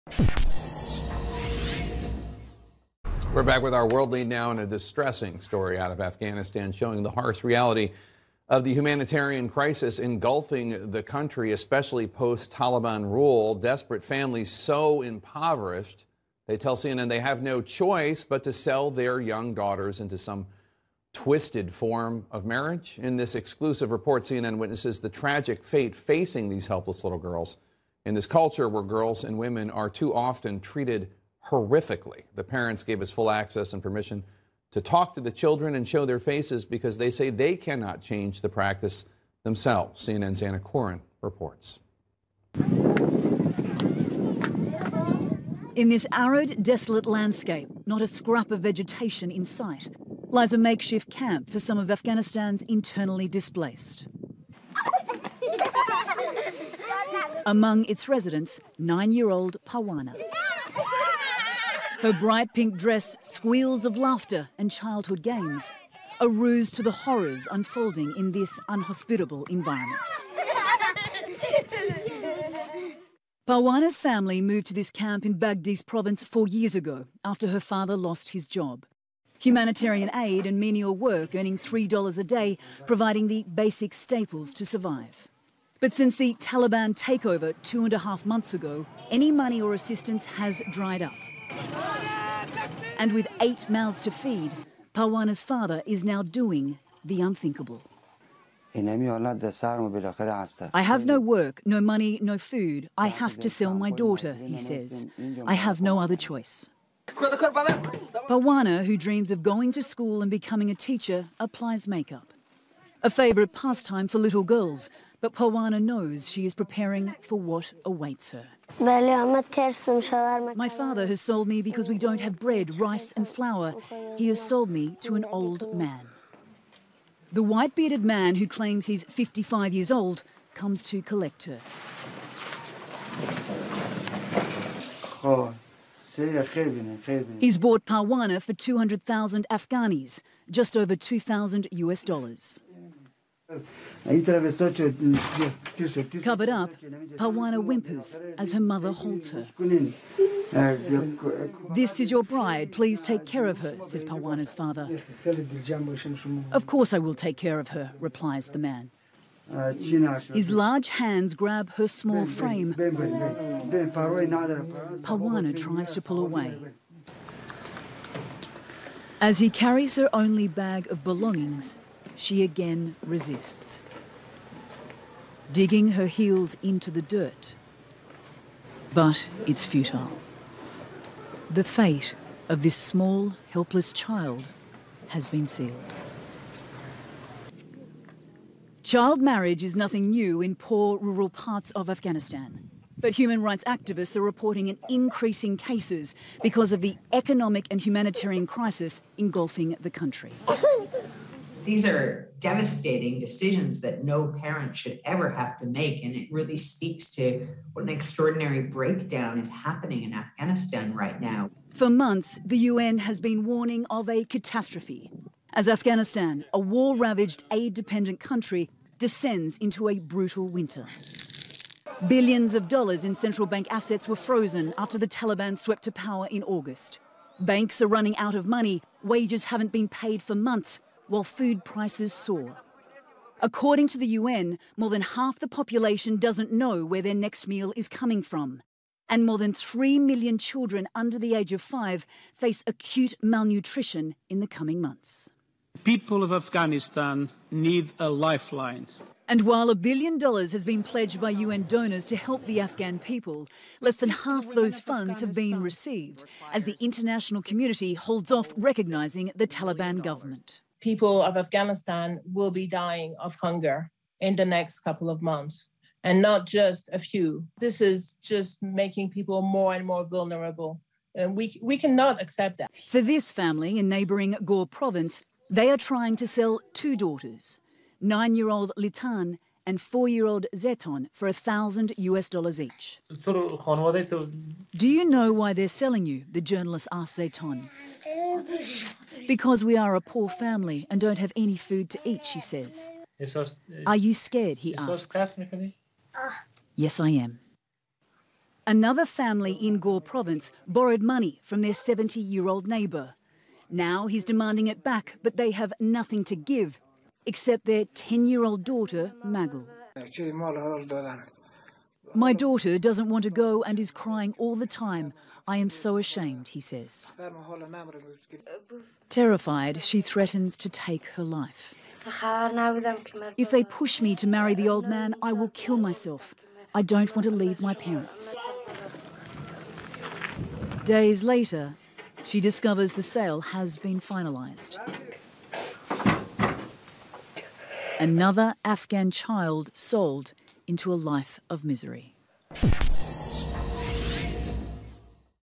Ils vendent leurs jeunes filles à des vieils hommes pour manger !
Vendre sa jeune fille contre de l'argent est commun en Afghanistan, mais avec l'arrivée des Talibans au pouvoir et une crise économique sans précédent, les tractations entre vieux pervers et familles pauvres ne se font plus sous le manteau, et les enfants sont vendus de plus en plus tôt. Un reportage CNN particulièrement choquant.